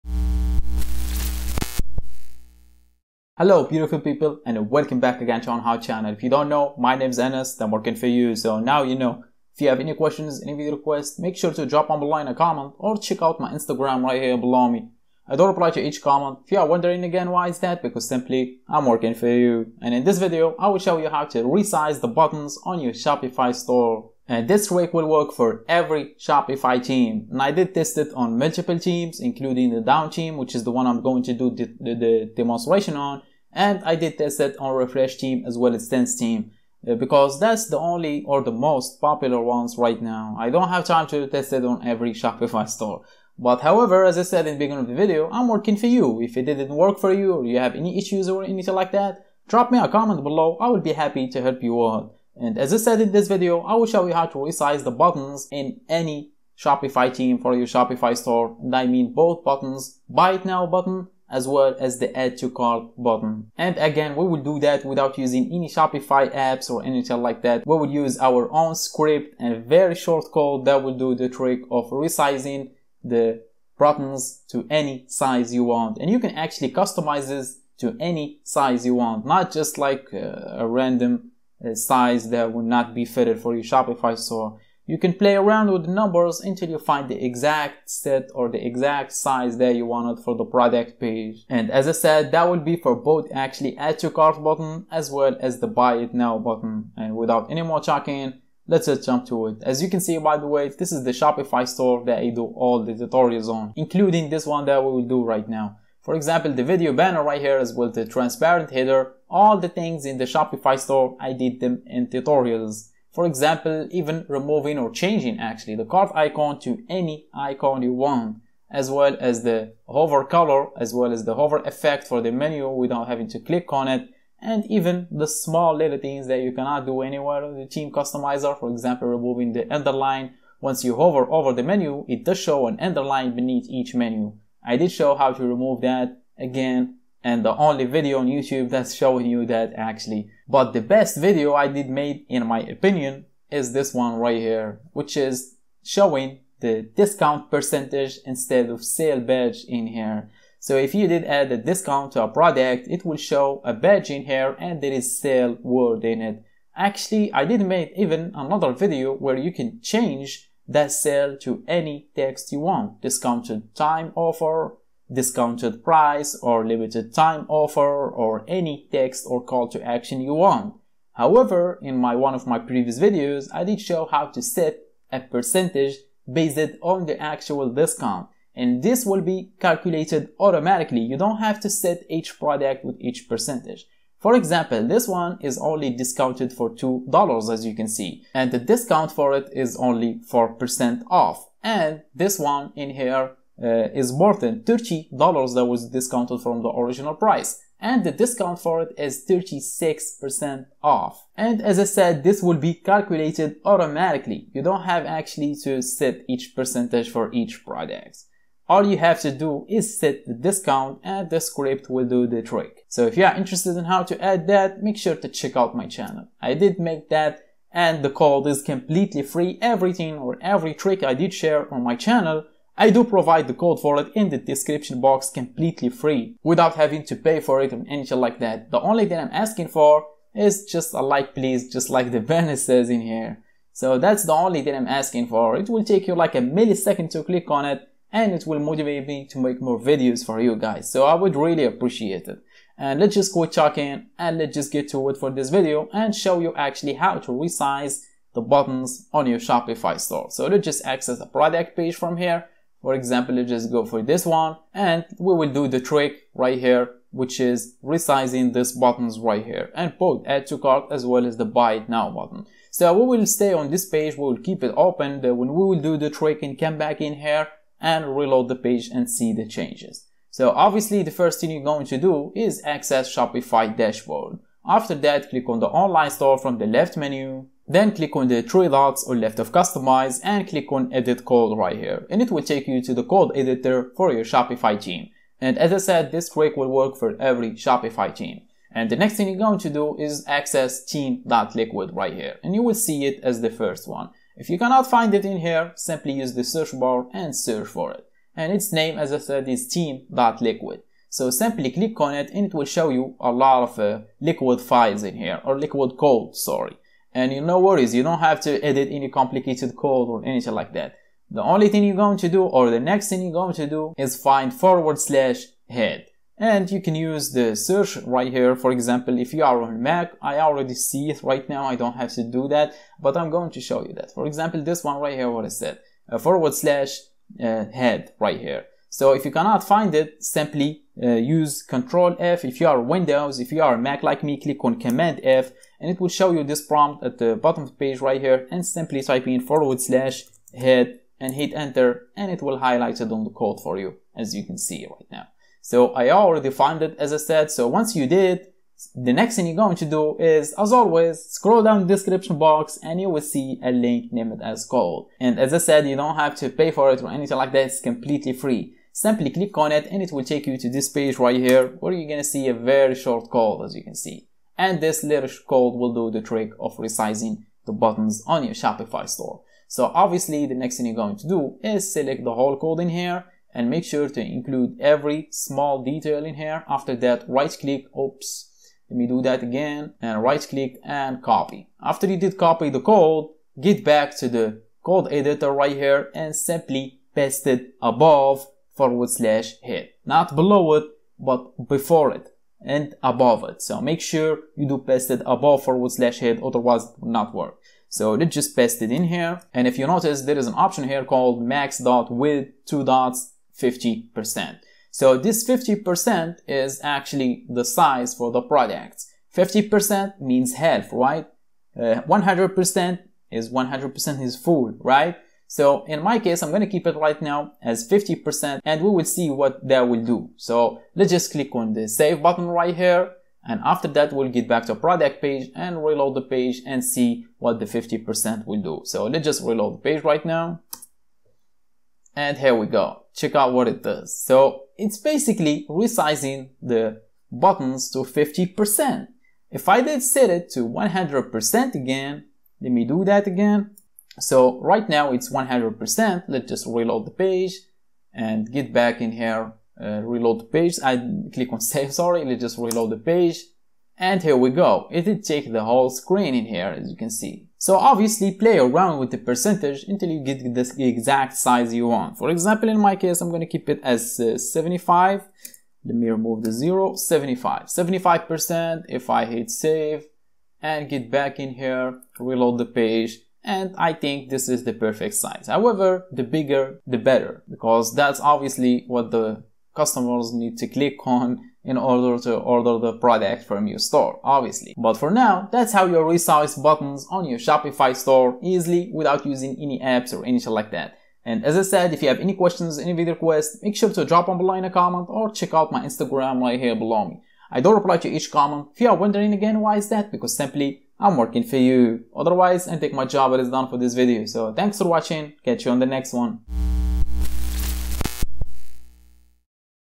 How To Change Buttons Size In Shopify
In this video i will show you how to change buttons size in Shopify and resize add to cart and buy it now buttons to make them either smaller or bigger.
Changing button sizes in Shopify is really important because buttons are an essential part of any Shopify store and it's important to make sure they're optimized for maximum usability.
By adjusting the size of your buttons, you can ensure that customers have a great user experience when shopping on your store, so it's important to choose a size that works best for each page or section on your Shopify store.
Larger buttons are often better at drawing attention than smaller ones, but if they’re too big then they may take up too much space or look out of place compared with other elements on the page.